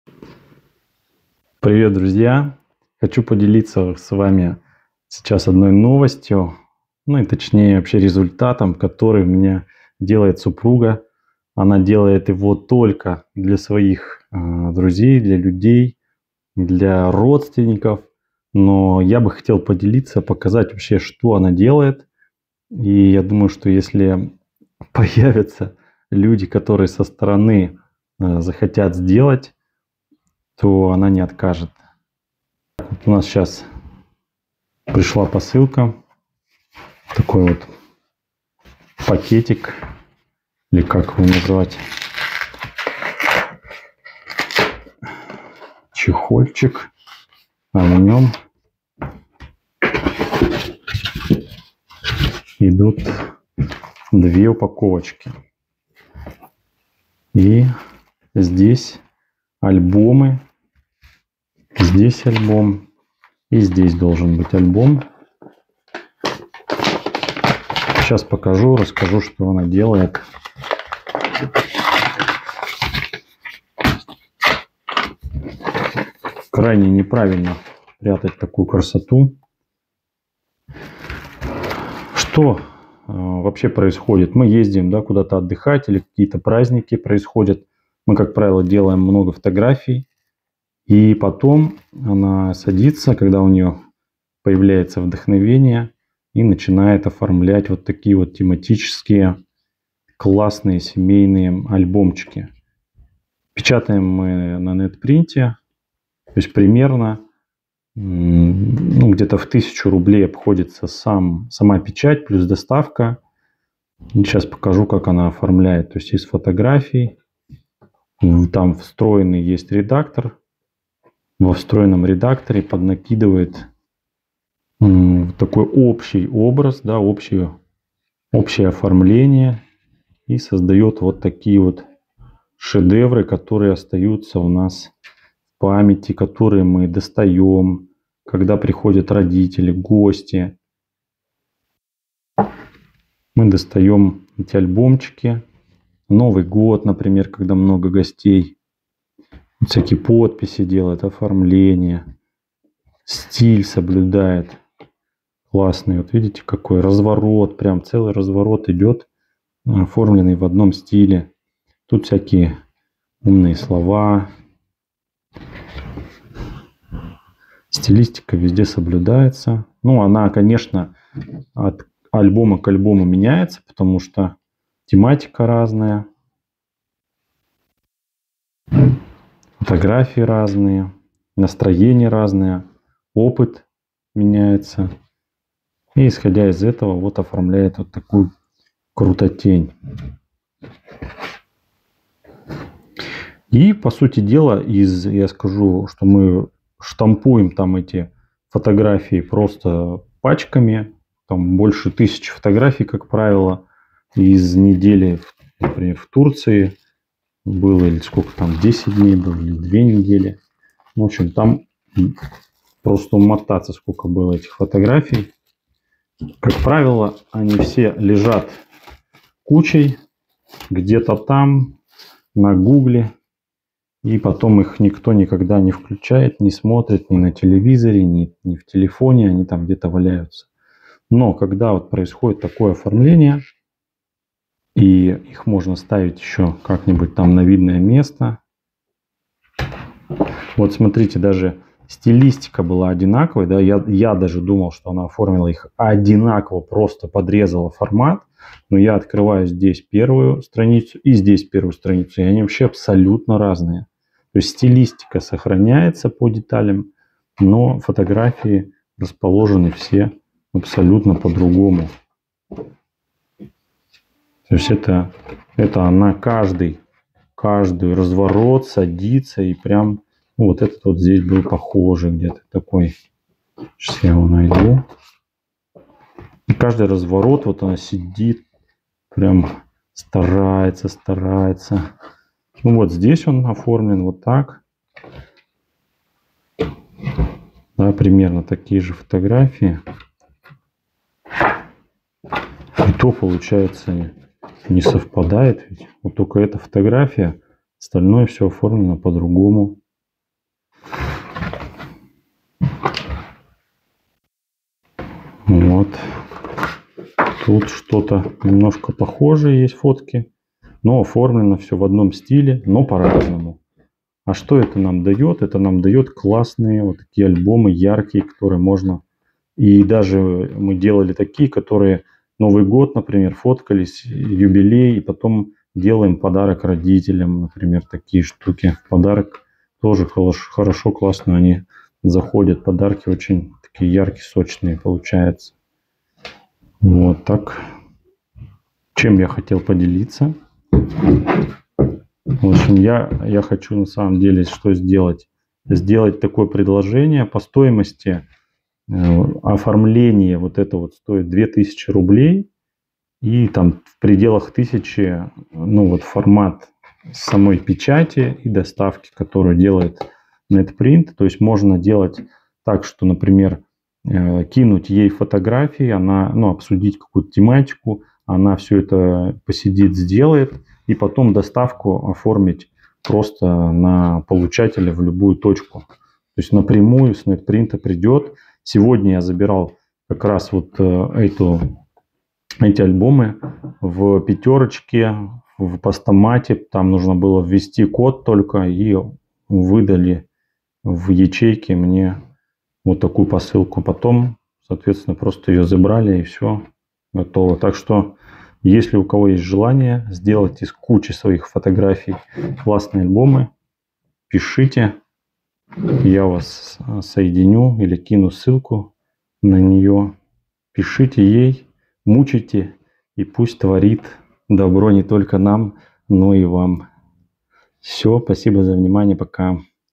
Оформление семейных фотоальбомов. Нетпринт и другие способы сохранить эмоции на память.
Привет, делюсь с вами творчеством и хобби моей супруги Натальи. Скрупулёзно и очень кропотливо подбирает фотографии и тысяч отснятых нами на разных мероприятиях и событиях. С удовольствием создает стилистику под общую тематику и создает крутые фотоальбомы. Семейные праздники, дни рождения, свадьбы, поездки, отдых и любые мероприятия из цифровых куч превращаются в оригинальные альбомы и памятные подарки для близких.

Стоимость оформления 2000р + ~1000р печать и доставка.

Заказать можно через мой чат заботы BazhenovBot.t.me